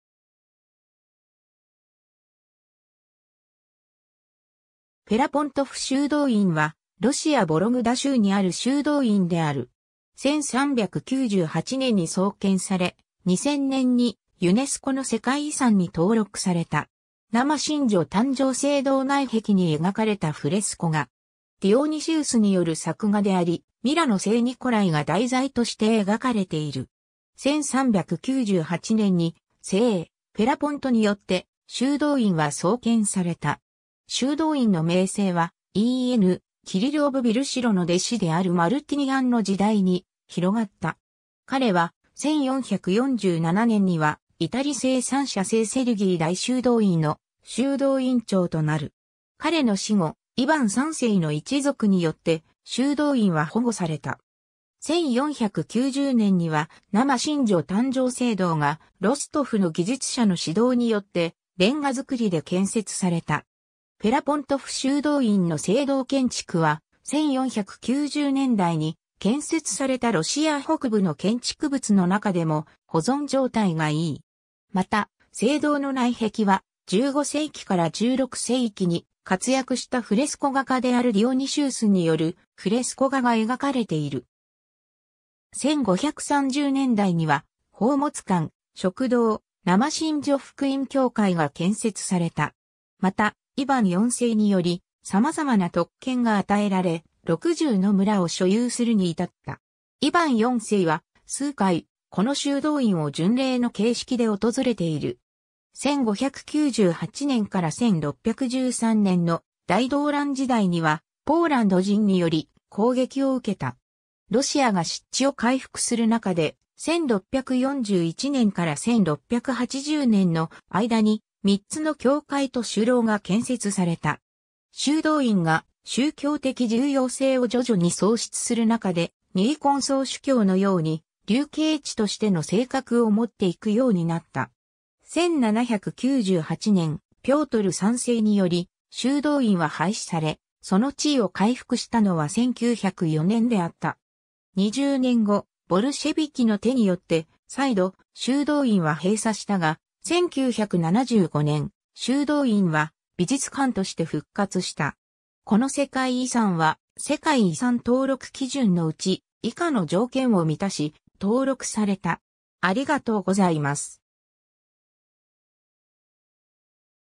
フェラポントフ修道院
フェラポントフ修道院（フェラポントフしゅうどういん、ロシア語:Ферапонтов монастырь）は、ロシア・ヴォログダ州にある修道院である。
1398年に創建され、2000年にUNESCOの世界遺産に登録された。
 生神女誕生聖堂内壁に描かれたフレスコ画。
ディオニシウスによる作画であり、ミラの聖ニコライが題材として描かれている。
 1398年に聖フェラポントによって、修道院は創建された。
修道院の名声は、en:Kirill of Beloozeroの弟子であるマルティニアンの時代に広がった。
彼は、1447年には、至聖三者聖セルギイ大修道院の修道院長となる。
 彼の死後、イヴァン3世の一族によって、修道院は保護された。
1490年には、生神女誕生聖堂が、ロストフの技術者の指導によって、レンガ造りで建設された。
フェラポントフ修道院の聖堂建築は、1490年代に建設されたロシア北部の建築物の中でも保存状態がいい。
また、聖堂の内壁は、15世紀から16世紀に活躍したフレスコ画家であるディオニシウスによる（en）フレスコ画が描かれている。
 1530年代には、宝物館、食堂、生神女福音教会が建設された。
また、イヴァン4世により、様々な特権が与えられ、60の村を所有するにいたった。
イヴァン4世は、数回、この修道院を巡礼の形式で訪れている。
 1598年から1613年の大動乱時代には、ポーランド人により、攻撃を受けた。
ロシアが失地を回復する中で、1641年から1680年の間に、3つの教会と鐘楼が建設された。
 修道院が宗教的重要性を徐々に喪失する中で、ニーコン総主教（en:Patriarch Nikon）のように、流刑地としての性格を持っていくようになった。
1798年、ピョートル3世により、修道院は廃止され、その地位を回復したのは1904年であった。
20年後、ボルシェヴィキの手によって、再度、修道院は閉鎖したが、1975年、修道院は、美術館として復活した。